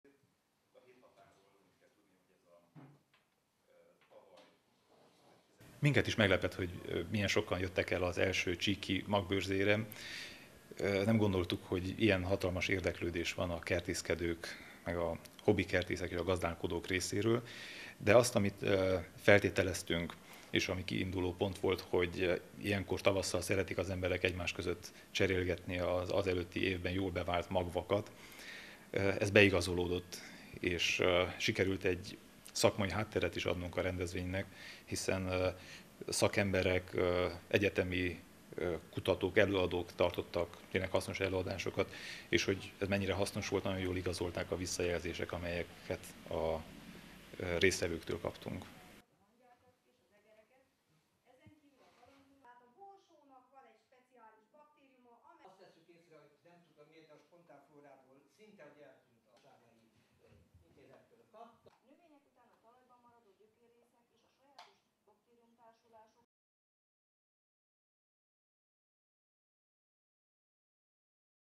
I. Csíki Magbörze
Népszerű és ennél fogva sikeres volt az I. Csíki Magbörze, amelyet 2013. február 23-án délelőtt tartottak a 7 határ gazdaoldal és a Sapientia EMTE szervezésében Csíkszeredában. A meghívott szakemberek érdeklődő és nagy számú hallgatóság előtt tartották meg előadásaikat.